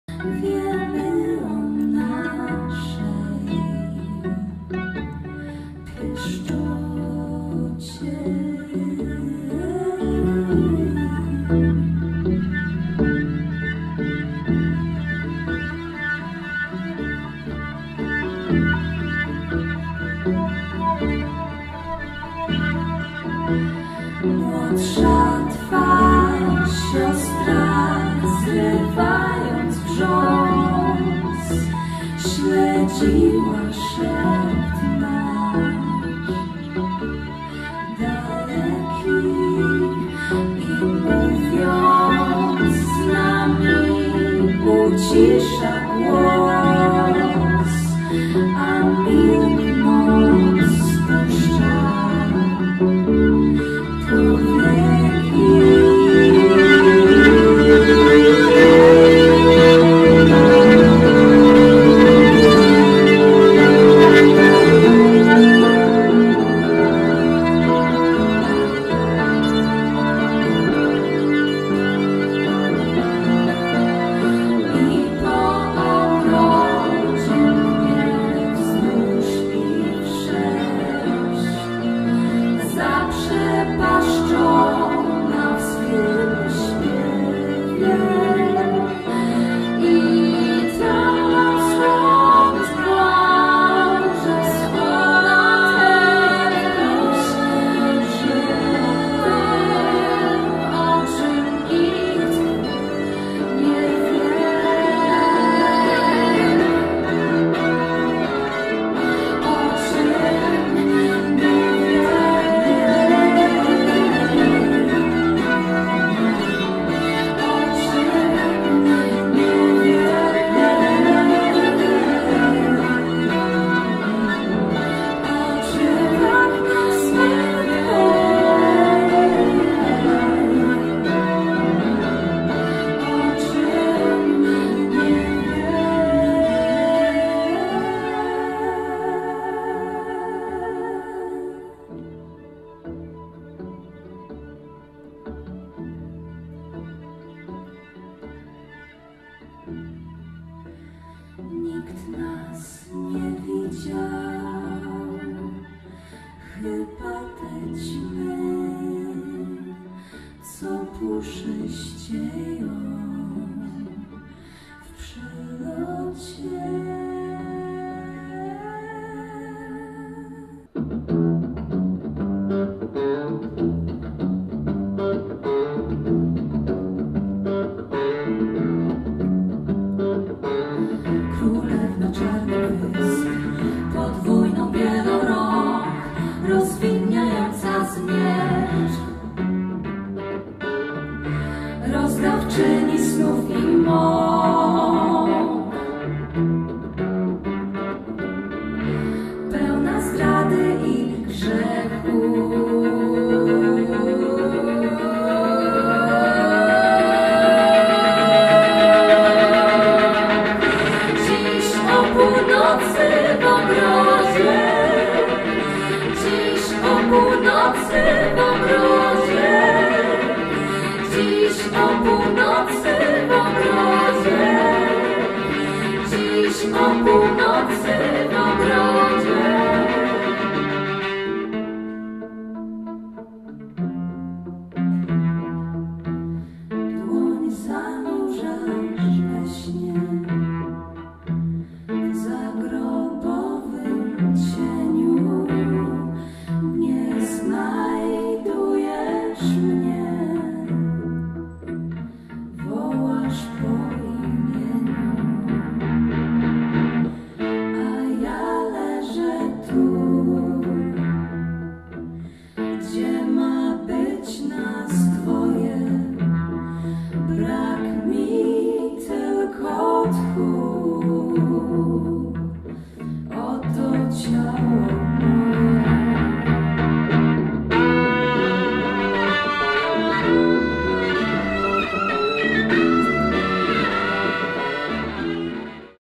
Trzy Dni Później - koncert jubileuszowy Studium Wokalnego
Koncert odbył się w MOK Głogów.